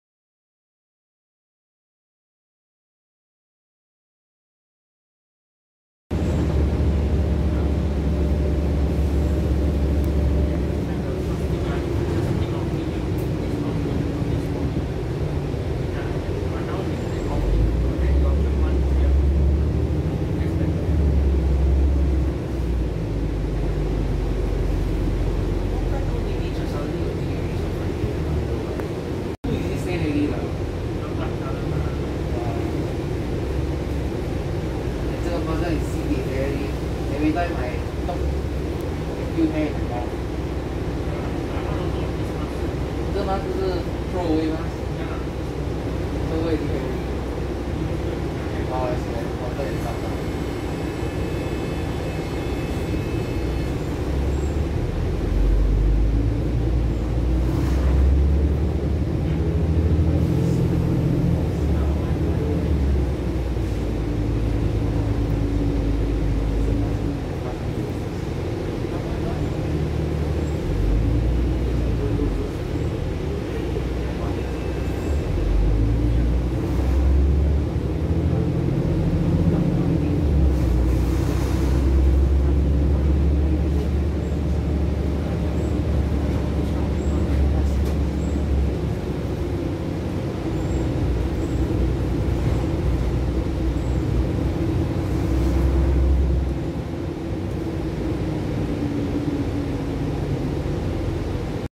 [SBST] SBS9889U on 186
TIB1222C films SBS9889U, the sole Volvo B10TL CDGE in the world on Service 186